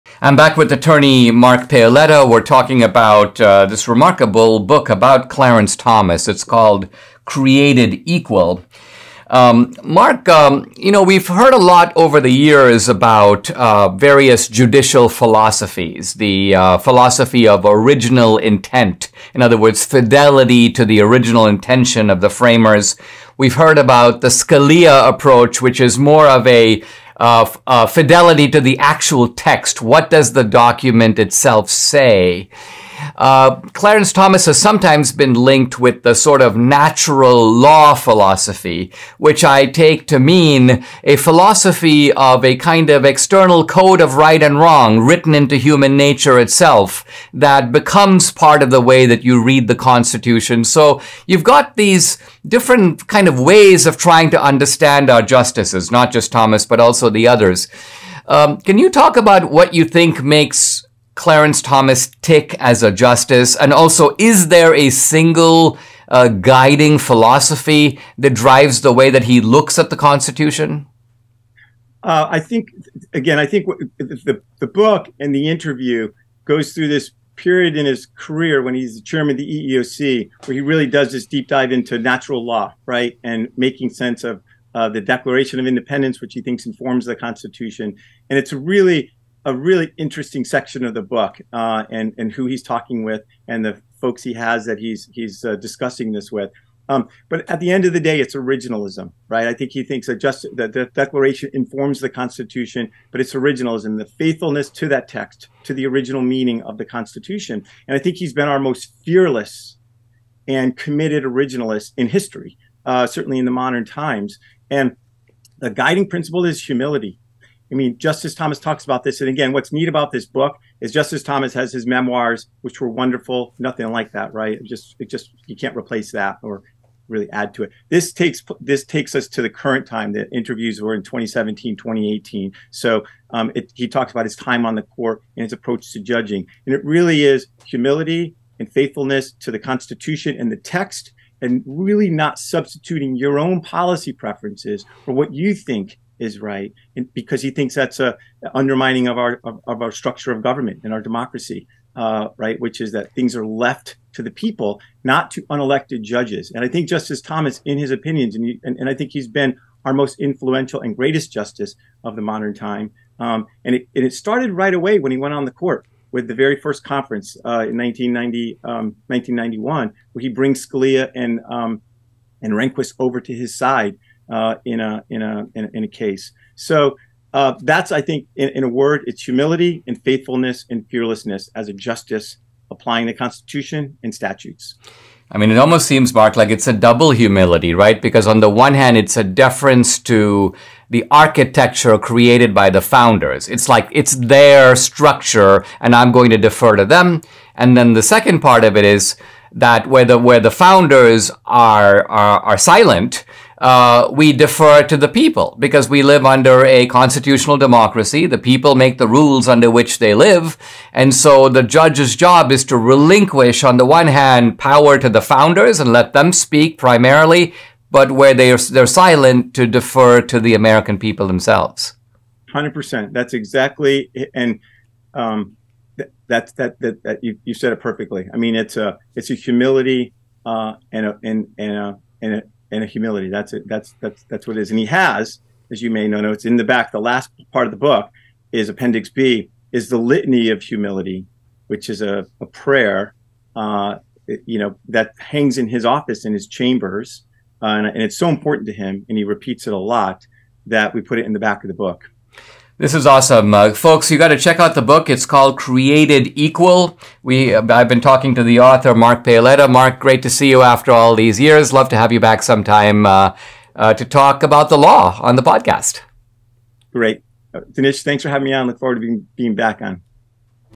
Mark Paoletta Joins Me to Discuss What Makes Clarence Thomas So Great
Author and attorney Mark Paoletta discusses the unique qualities of Clarence Thomas that make him the greatest living justice.